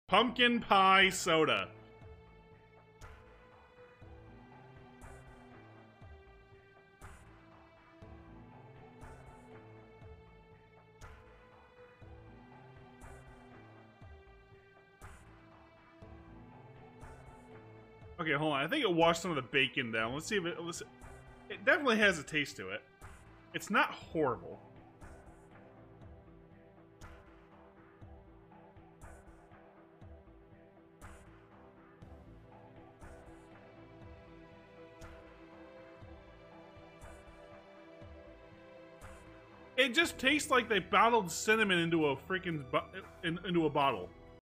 Pumpkin Pie??? Soda
Clip of Trax trying Pumpkin Pie Soda!